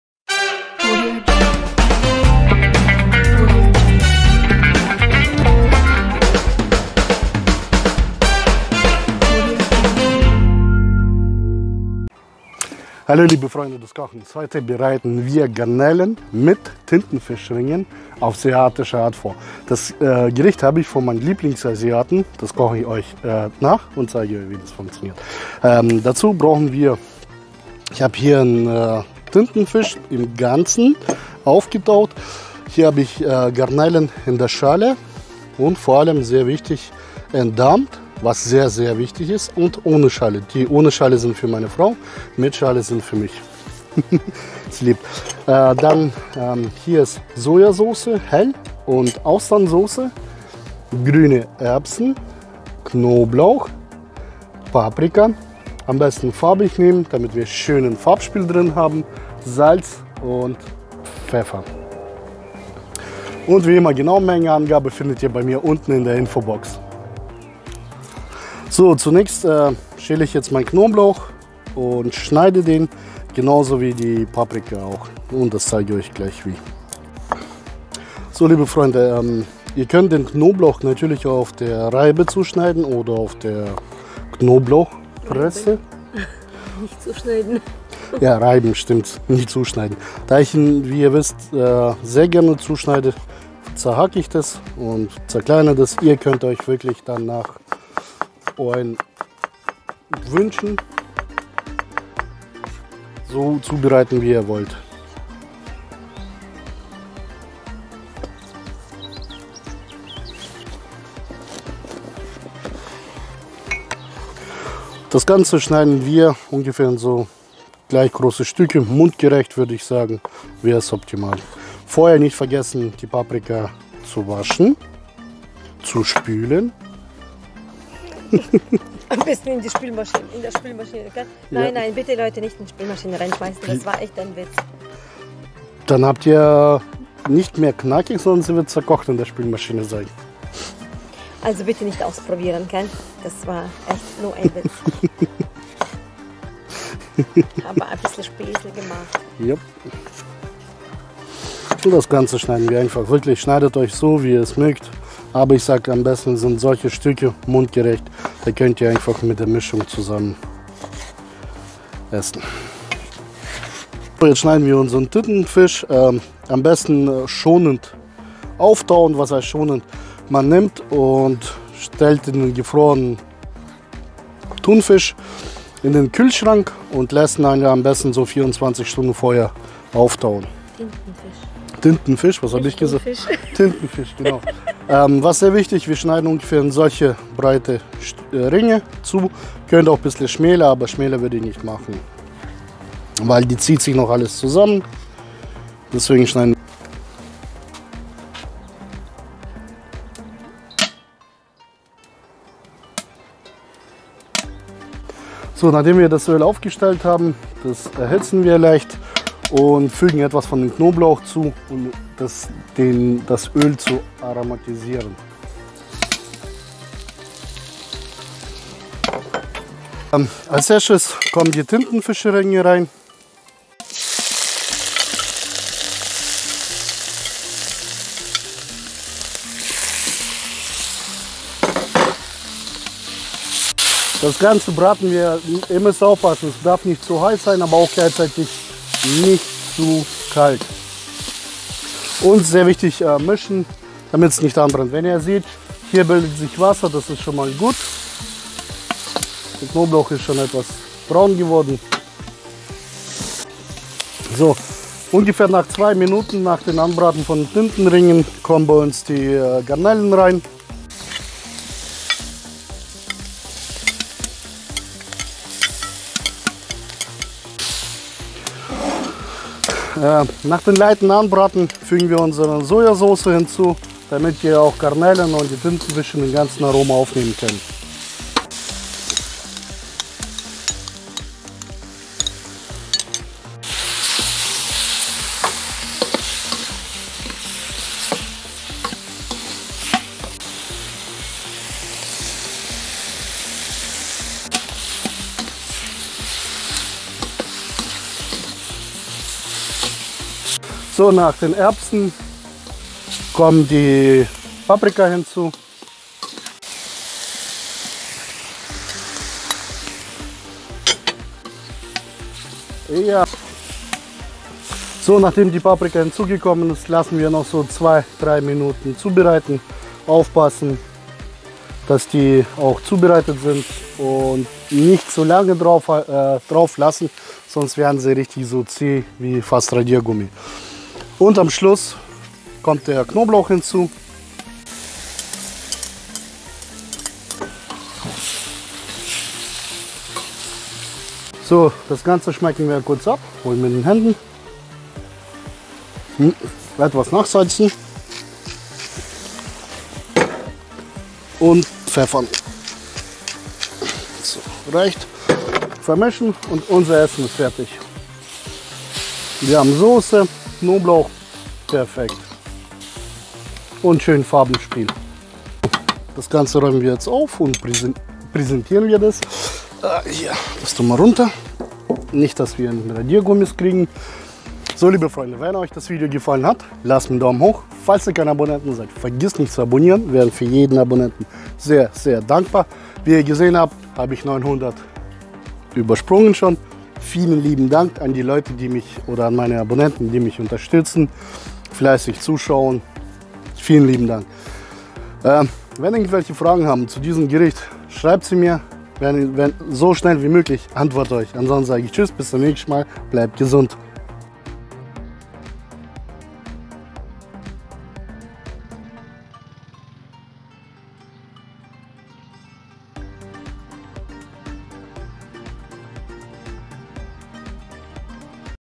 Garnelen mit Tintenfisch Ringe auf asiatische Art
Das Rezept ist sehr leicht und lecker. Probiert es aus, ihr werdet es lieben! :-)

Zutaten:
500 g Tintenfisch
500 g Garnelen / mit Schale
2 Paprikas rote gelbe
150 g grüne Erbsen TK Ware
2-6 Knoblauch Zehen nach Geschmack
3-5 El Austernsauce
3-5 El Sojasauce

Gewürze:
Salz
Pfeffer

Alle Namen und Bezeichnungen sind geschützt aber ein Bestandteil des Gerichtes. Dieses Video kann Werbung beinhalten